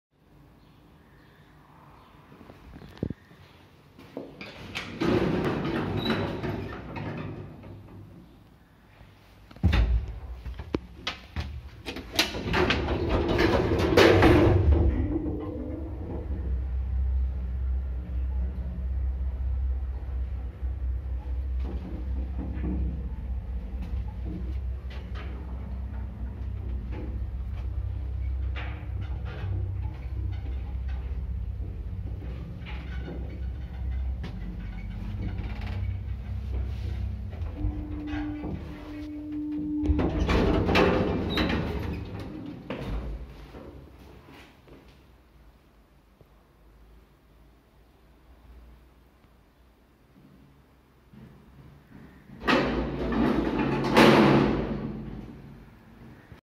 Лифт МЛЗ 1990 г.в., V=0,71 м/с, г/п 400 кг
Город Вологда, Улица Дальняя, дом 20, подъезд 6.